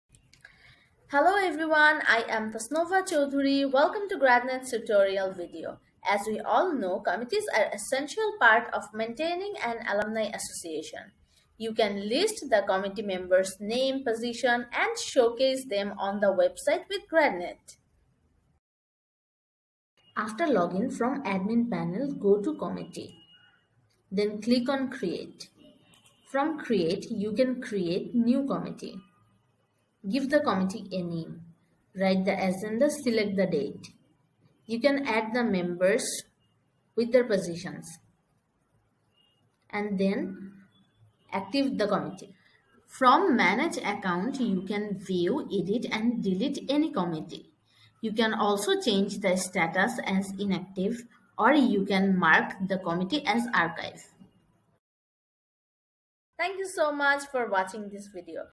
Manage your alumni association's committees & sub-committee with Gradnet
1. Create & manage committee
2. Active committee
3. Committee archive
4. Add members
5. Committee position management

Chapters:
0:00 Introduction
0:19 Create a new committee
0:42 Manage the committee
0:56 Finish